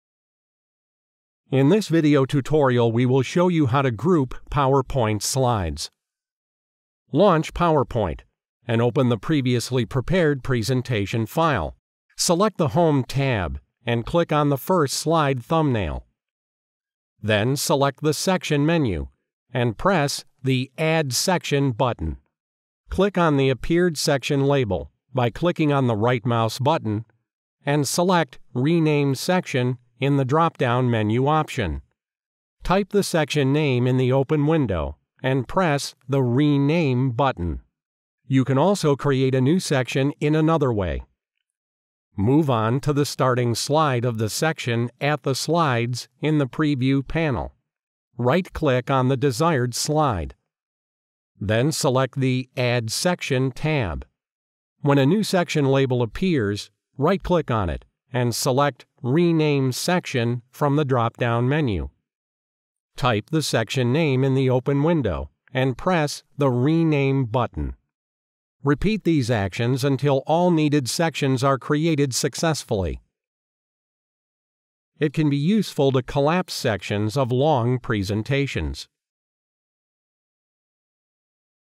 How to Group PowerPoint 2010 Slides
Learn how to group slides in Microsoft PowerPoint.

In this video tutorial we will show you how to group PowerPoint slides.

Launch the PowerPoint and open the previously prepared presentation file. Select "Home" tab and click on the first slide thumbnail. Then select the "Section" menu and press on the "Add Section" button.

Click on the appeared section label by clicking the right mouse button and select "Rename Section" in the dropdown menu option. Type the section name in the opened window and press "Rename" button.

You can also create a new section in another way:

Move on to the starting slide of the section at the slides in the preview panel. Right clicking on the desired slide. Then select the "Add Section" tab.

When a new section label appears, right click on it and select "Rename Section" option from the dropdown menu. Type the section name in the opened window and press "Rename" button.

Repeat these actions until all needed sections are created successfully. It can be useful to collapse sections of long presentations.